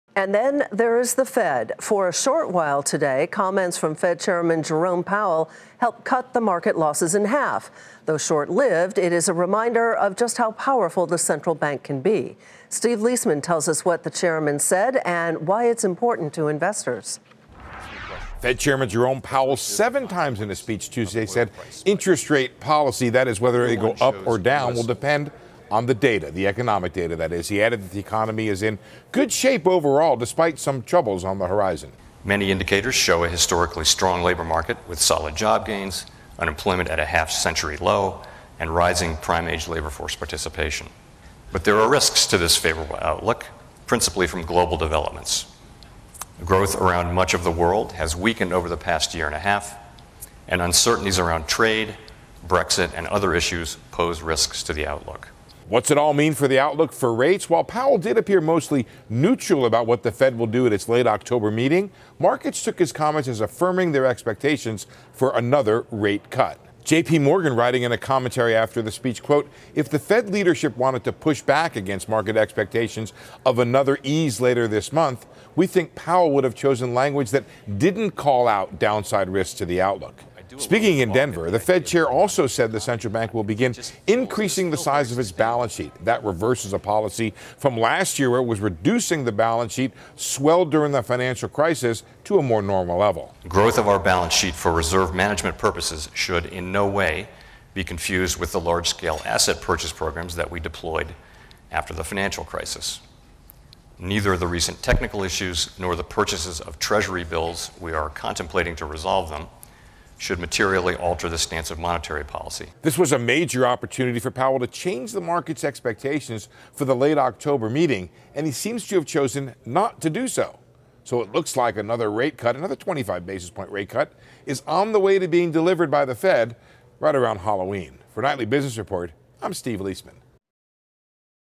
Federal Reserve’s Jerome Powell: Data Dependent
Steve Liesman reports on the latest comments Federal Reserve Chairman Jerome Powell made and why it’s important to investors.